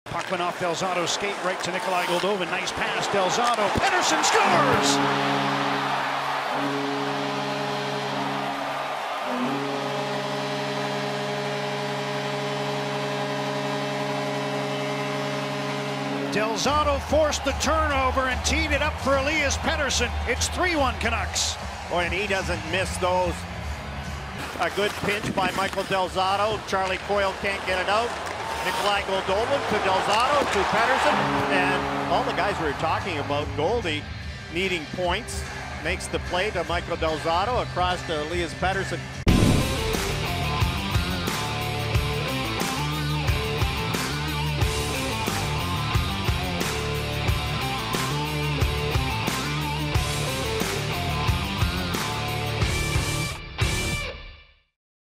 GOTTA SEE IT: Michael Del Zotto Forces Turnover, Feeds Elias Pettersson For Lethal One-Timer
Watch as Michael Del Zotto forces a turnover the Minnesota Wild zone and then feeds Elias Pettersson who beats Devan Dubnyk with a lethal one-timer.